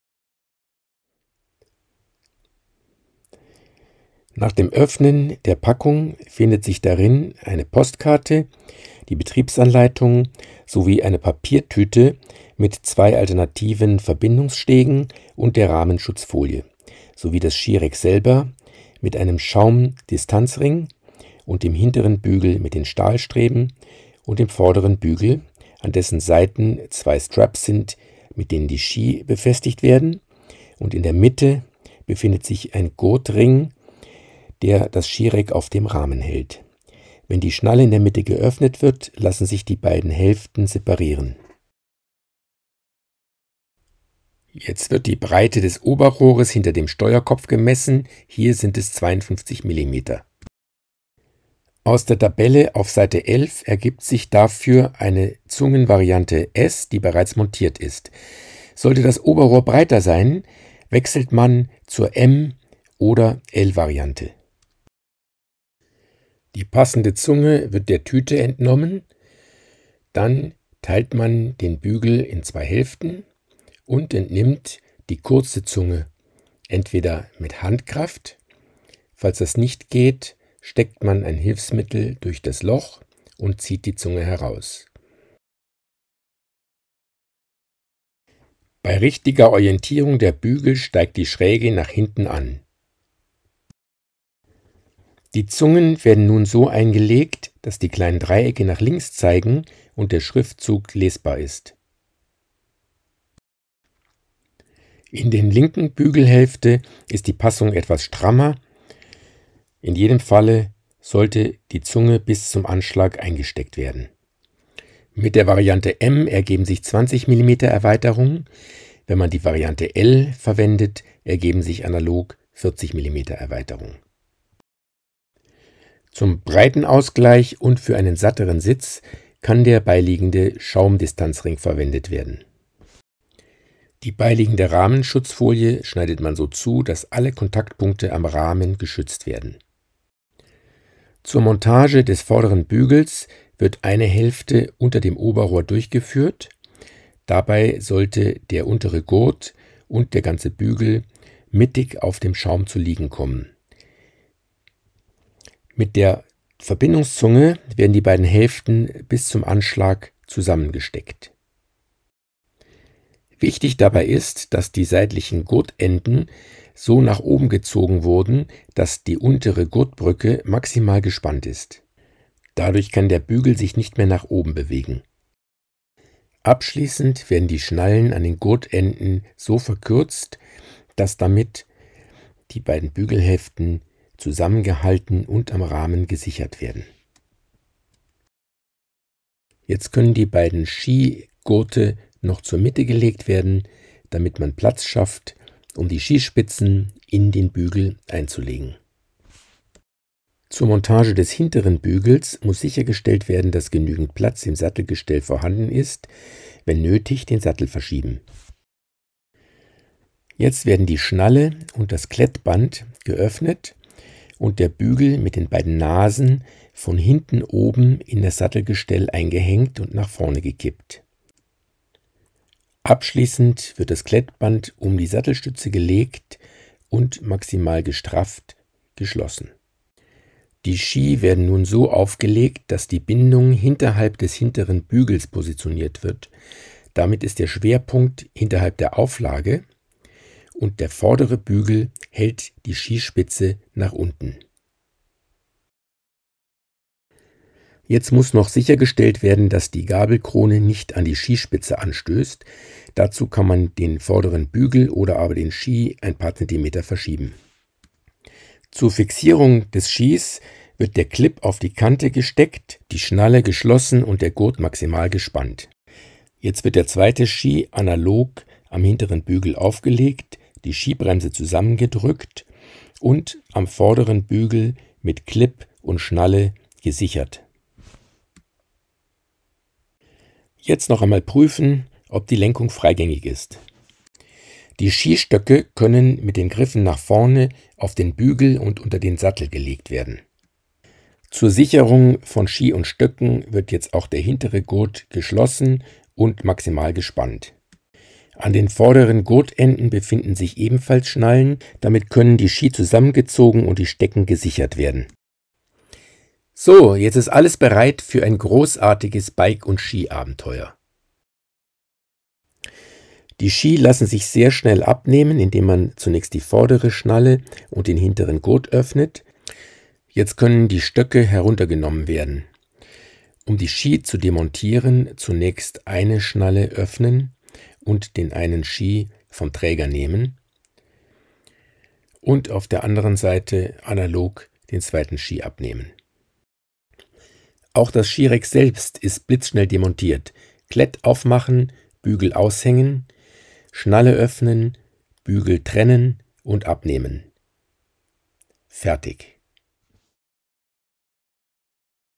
SKI RACK / 03 - Skihalterung - Montage
Alle Informationen zur Montage des SKI RACK / 03 - das Produkt zur Befestigung von Ski oder Splitboards am Rad.

Mit dem SKI RACK / 03 eröffnen sich neue Möglichkeiten für Skitourengeher*innen (und Splitboardnutzer*innen). Ob die gesamte Anreise zur Skitour umweltfreundlich mit dem Fahrrad, die Abkürzung einer langwierigen Forststraße, das Pedalieren in ein abgelegenes Tal oder das Erreichen der Schneegrenze auf möglicherweise noch gesperrten Straßen – der Aktionsradius wird dank der Kombination aus Ski und Bike deutlich vergrößert und ermöglicht eindrückliche Naturerlebnisse.

00:00 Unboxing
00:44 Messung der Oberrohr-Breite
00:51 Ermittlung der passenden Verbindungs-Zunge
01:05 Austausch der Verbindungs-Zunge
02:13 Hinweis zur Rahmenschutzfolie
02:20 Befestigung des vorderen Bügels
02:55 Sicherung der Bügelhälften
03:20 Befestigung des hinteren Bügels
03:50 Auflegen der Ski
04:07 Gabelkrone und  Skispitzen
04:18 Fixierung der Ski vorne
04:40 Freie Lenkung
04:44 Befestigung der Skistöcke
05:05 Abfahrbereit!
05:12 Abnehmen der Skiausrüstung
05:36 Demontage des SKI RACKs